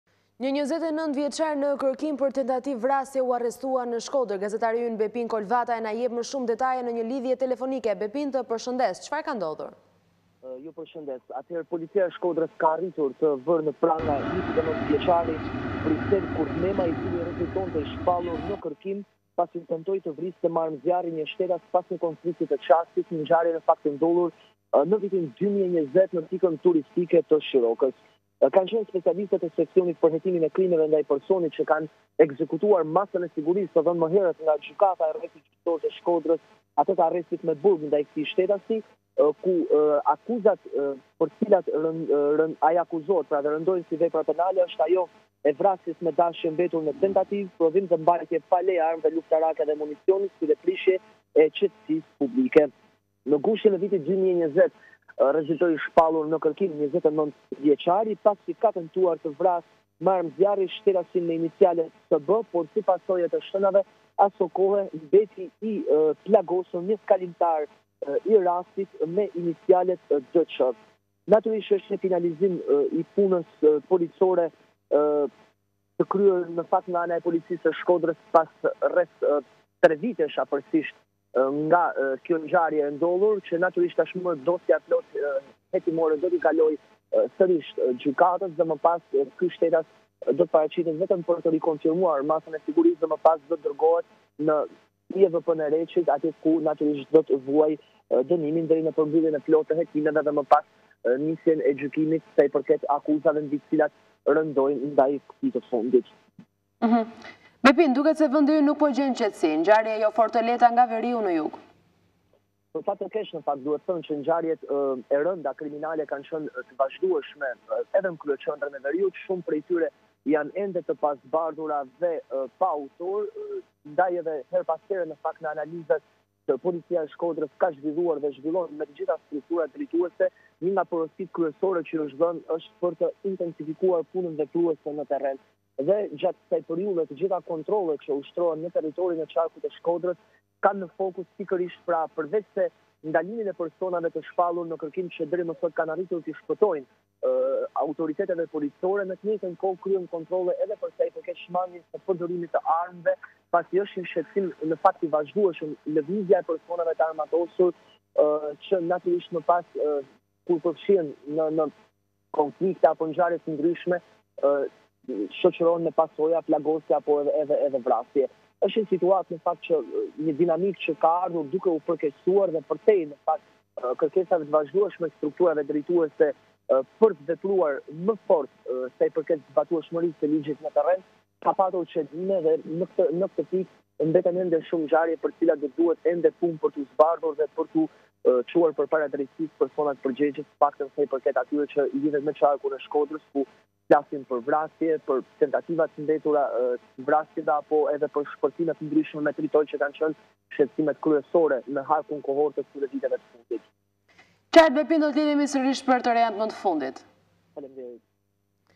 I shpallur në kërkim për tentativë vrasjeje dhe plagosje, arrestohet 29-vjeçari në Shkodër
Policia e Shkodrës ka arritur të vërë në pranga 29-vjeçarin e shpallur në kërkim, që tentoi të vriste me armë zjarri, një shtetas.

Bëhet fjalë për Brisel Kurtmetajn, i cili pas një konflikti të çastit tentoi të vriste një shtetas, ku si pasojë e shkrepjes së armëve do të mbetej i plagosur një shtetas i pafajshëm, kalimtar i rastit, ngjarje kjo e ndodhur në vitin 2020.

“Specialistët e Seksionit për Hetimin e Krimeve ndaj Personit ekzekutuan masën e sigurisë “Arrest në burg”, të caktuar nga Gjykata e Rrethit Gjyqësor Shkodër, për veprat penale “Vrasja me dashje” mbetur në tentativë, “Prodhimi dhe mbajtja pa leje e armëve luftarake dhe e municionit” dhe “Prishja e qetësisë publike”, për shtetasin:

-B. K.(G.), 29 vjeç, banues në lagjen “Guerrile”, Shkodër.

Ky shtetas ishte shpallur në kërkim pasi në muajin gusht të vitit 2020, pas një konflikti të çastit, ka tentuar të vrasë me armë zjarri shtetasin S. B. Si pasojë e të shtënave është plagosur lehtë shtetasi D. Q., kalimtar i rastit.

Materialet procedurale i kaluan Prokurorisë pranë Gjykatës së Rrethit Gjyqësor Shkodër, për veprime të mëtejshme”, thuhet në njoftimin e policisë së Shkodrës.